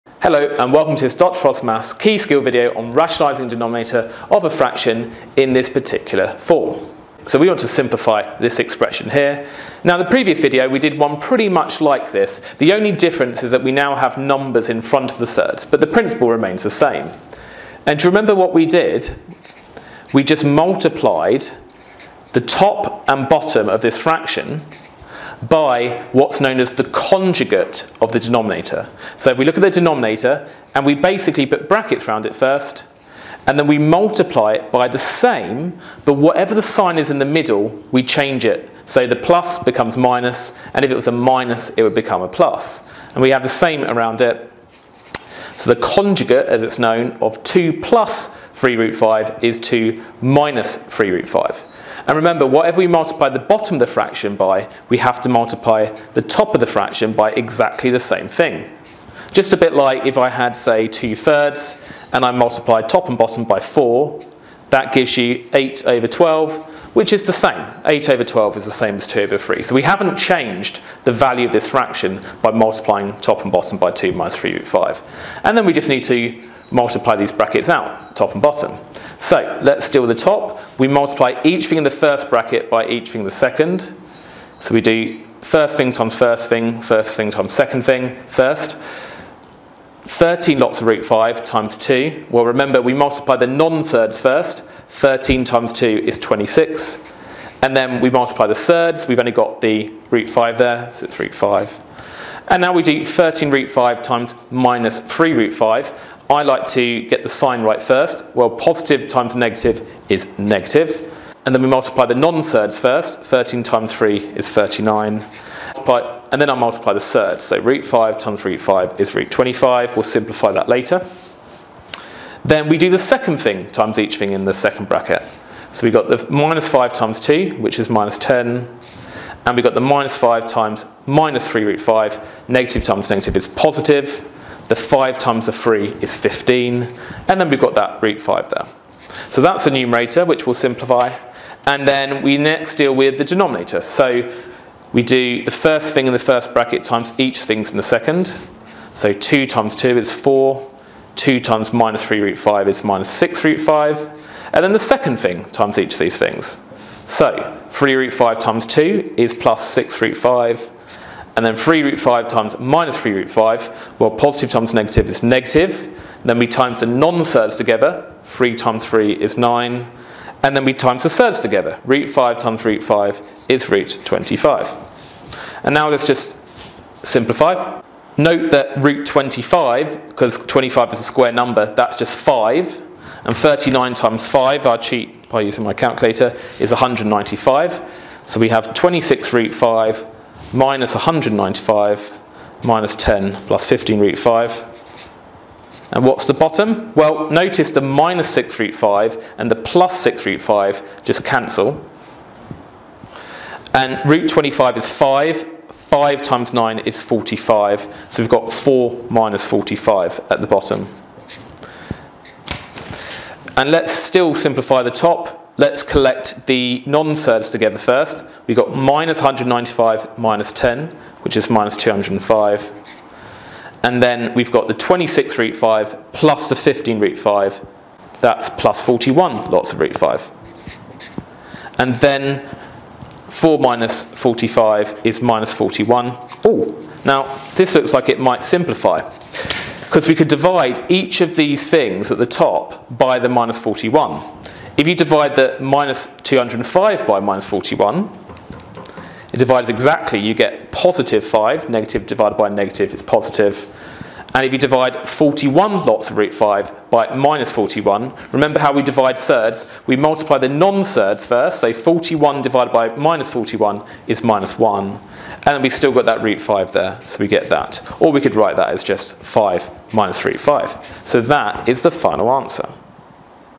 Key Skill - Rationalise the denominator of a fraction in the form (a+b√c)(d+e√c).
"Rationalise the denominator of a fraction in the form (a+b√c)(d+e√c)."